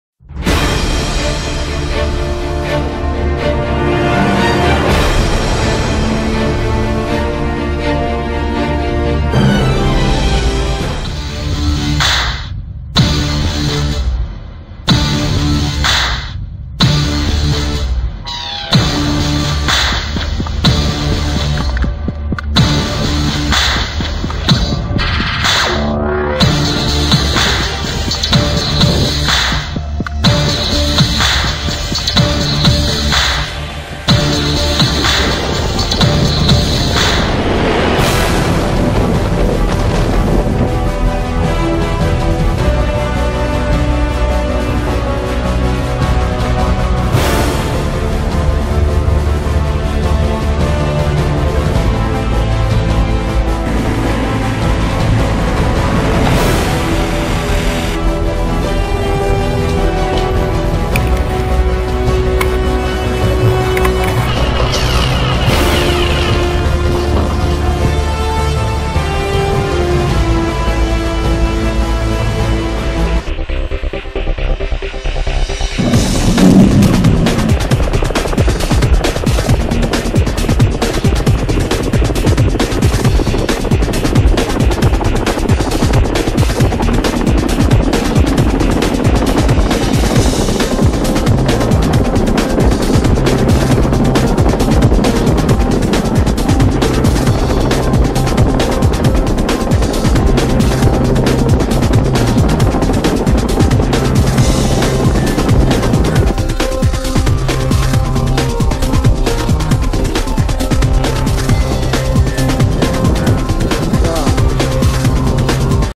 Barracuda S A R capabilities demonstration video
Barracuda S A R  capabilities demonstration video during winter storms up to Force 10 and 6m wave heights